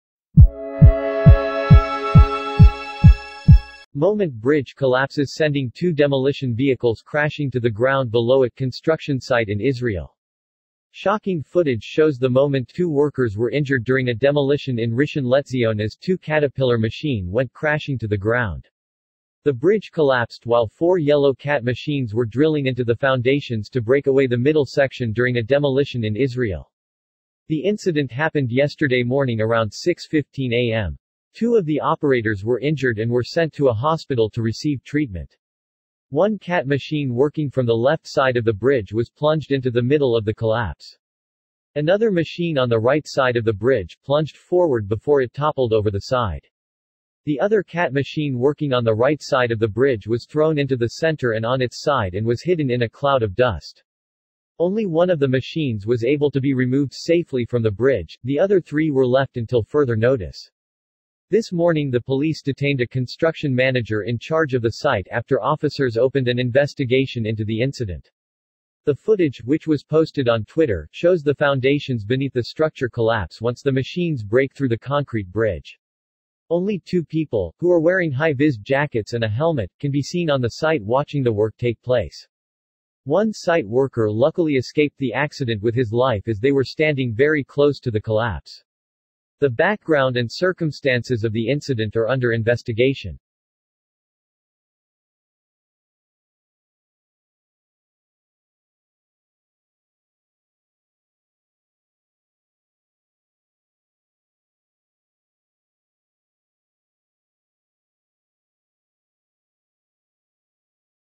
Shocking moment tractors overturns as bridge collapses during deconstruction in Israel
Shocking footage shows the moment two workers were injured during a demolition in Rishon Lezion as two Caterpillar machine went crashing to the ground.